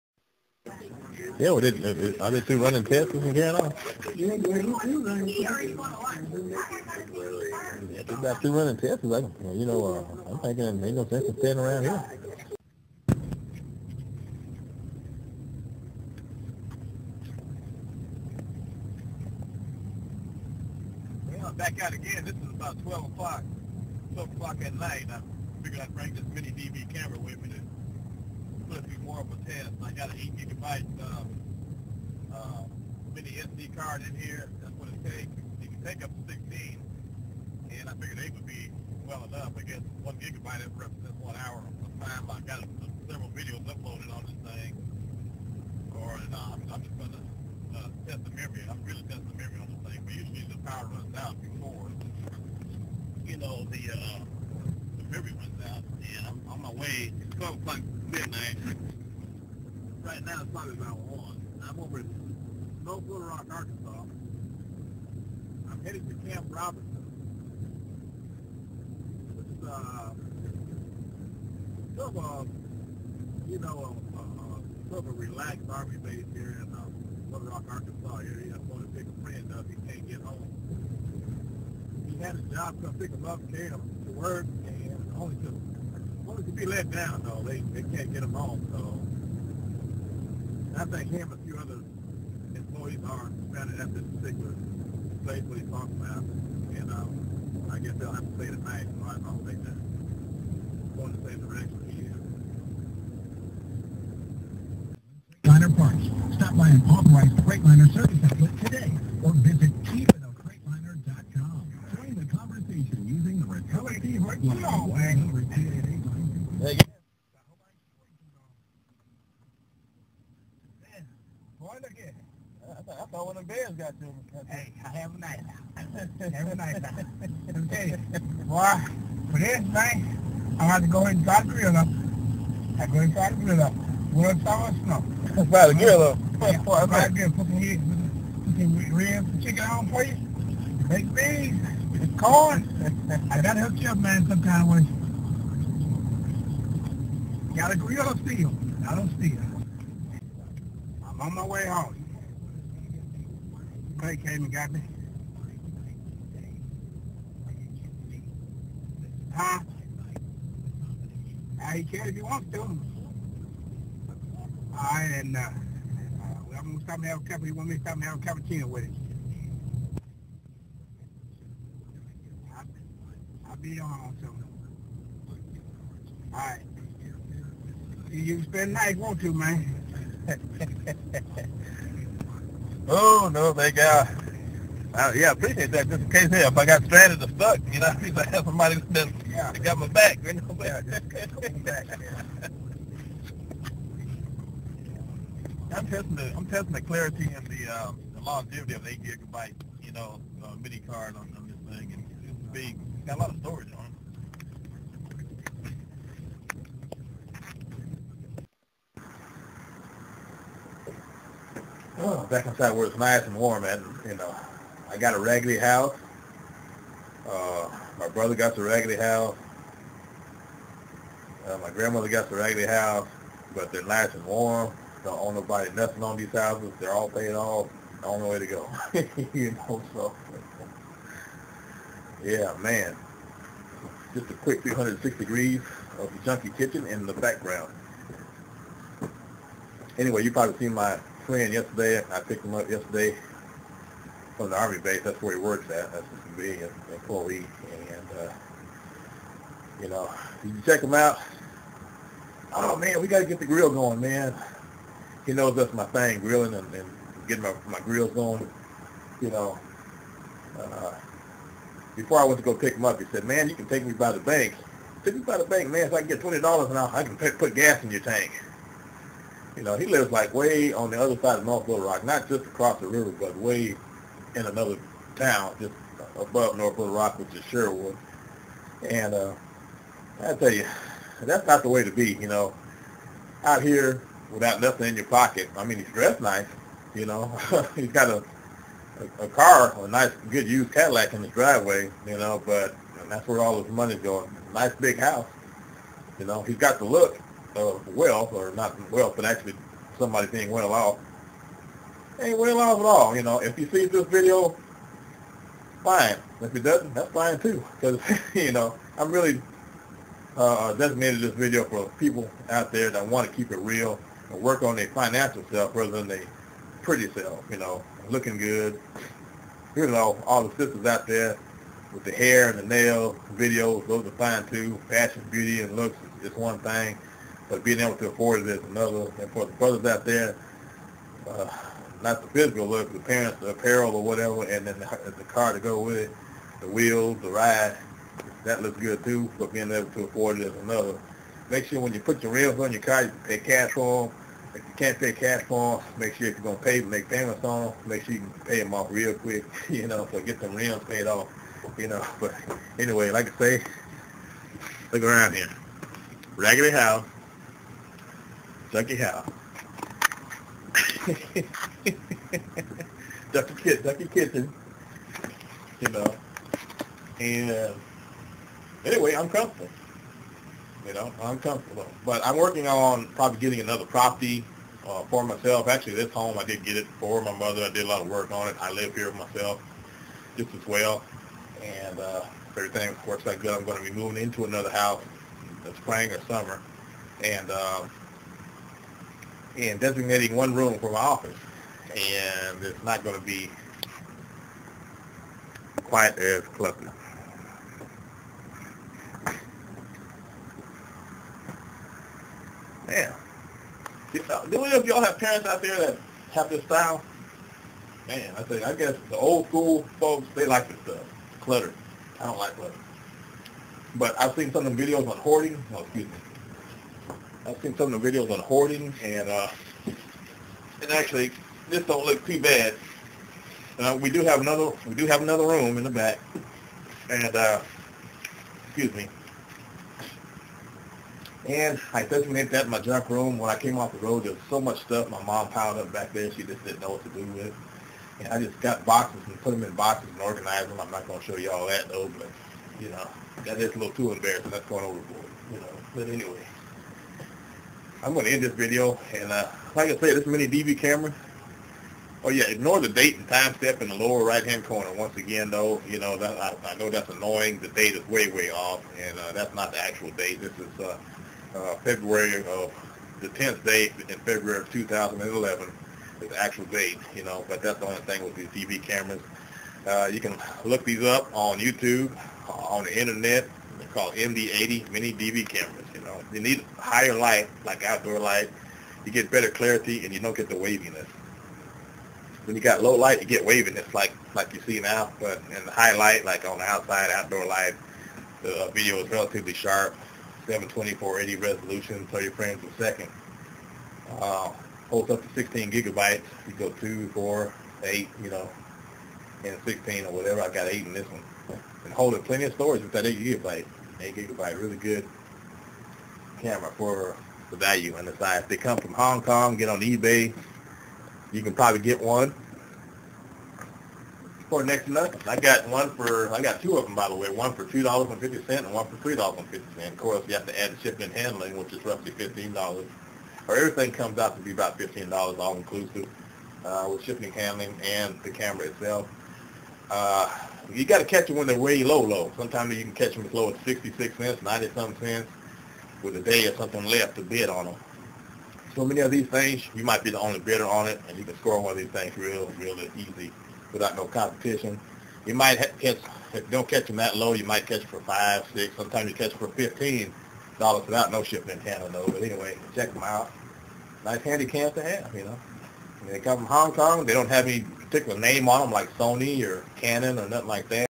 MD80 Mini Spy Cam
I guess this concludes this test of this user friendly video cam.  I pulled two of them off of Ebay for $5.00 all together $30.00+ with shipping.